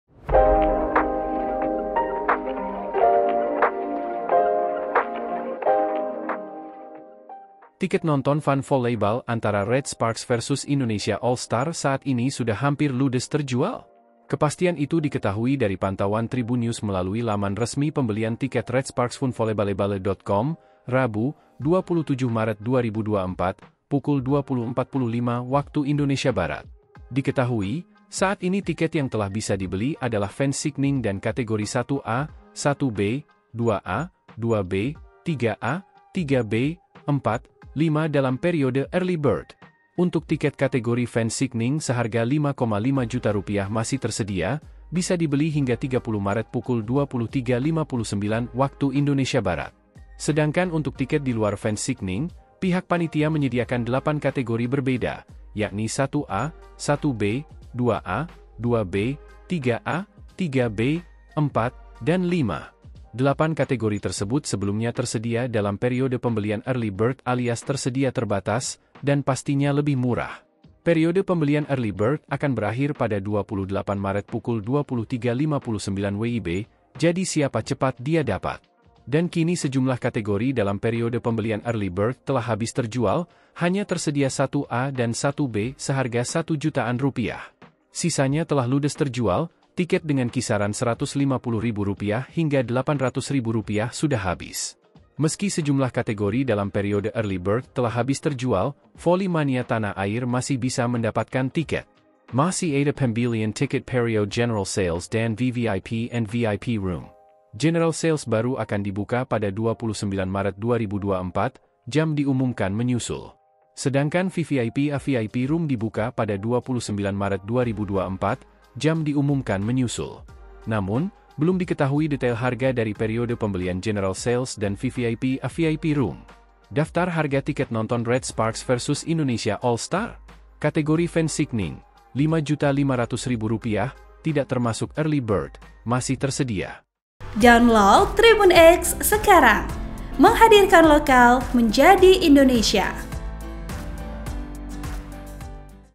Tiket Nonton Red Sparks vs Indonesia All Star HAMPIR LUDES TERJUAL, Tinggal Sisa Harga Rp 1 Jutaan
TRIBUN-VIDEO.COM - Tiket nonton Fun Volleyball antara Red Sparks vs Indonesia All Star saat ini sudah hampir ludes terjual.

Kepastian itu diketahui dari pantauan Tribunnews melalui laman resmi pembelian tiket redsparksfunvolleyball.com, Rabu (27/3/2024) pukul 20.45 WIB.

Diketahui, saat ini tiket yang telah bisa dibeli adalah fans signing dan kategori 1A, 1B, 2A, 2B, 3A, 3B, 4, 5 dalam periode early bird.

Untuk tiket kategori fans signing seharga Rp5,5 juta masih tersedia, bisa dibeli hingga 30 Maret pukul 23.59 WIB.

Sedangkan untuk tiket diluar fans signing, pihak panitia menyediakan delapan kategori berbeda, yakni 1A, 1B, 2A, 2B, 3A, 3B, 4, dan 5.

Delapan kategori tersebut sebelumnya tersedia dalam periode pembelian early bird alias tersedia terbatas, dan pastinya lebih murah.

Periode pembelian early bird akan berakhir pada 28 Maret 23.59 WIB, jadi siapa cepat dia dapat.

Dan kini sejumlah kategori dalam periode pembelian early bird telah habis terjual, hanya tersedia 1A dan 1B seharga Rp1 jutaan.

Sisanya telah ludes terjual, tiket dengan kisaran Rp150 ribu hingga  Rp800 ribu sudah habis.

Meski sejumlah kategori dalam periode early bird telah habis terjual, voli mania Tanah Air masih bisa mendapatkan tiket.

Masih ada pembelian tiket periode General Sales dan VVIP & VIP ROOM

General Sales baru akan dibuka pada 29/03/2024, jam diumumkan menyusul

Sedangkan VVIP & VIP ROOM dibuka pada 29/03/2024, jam diumumkan menyusul

Namun, belum diketahui detail harga dari periode pembelian General Sales dan VVIP & VIP ROOM.

Daftar Harga Tiket Nonton Red Sparks vs Indonesia All Star
Kategori Fan Signing: Rp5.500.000 (Tidak termasuk early bird) - masih tersedia

(Periode Early Bird)

1. Kategori 1A: Rp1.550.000 - masih tersedia

2. Kategori 1B: Rp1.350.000 - masih tersedia

3. Kategori 2A: Rp800.000 - habis

4. Kategori 2B: Rp650.000 - habis

5. Kategori 3A: Rp475.000 - habis

6. Kategori 3B: Rp375.000 - habis

7. Kategori 4: RP250.000 - habis

8. Kategori 5: Rp150.000 - habis

Note:

- Link pembelian: klik di sini

- Jumlah tiket early bird terbatas pada section tertentu.

- 1 (satu) tiket berlaku untuk menonton dua pertandingan Fun Volleyball.

- Maksimal 2 (dua) tiket dalam satu kali transaksi pada kategori yang sama.

- Harga belum termasuk Pajak Pemerintah 10 persen, jasa admin platform, dan convenience fee.

(Tribunnews.com/Isnaini)

Artikel ini telah tayang di Tribunnews.com dengan judul Tiket Nonton Red Sparks vs Indonesia All Star Hampir Ludes Terjual, Tinggal Sisa Harga Rp1 Jutaan,
Penulis: Isnaini Nurdianti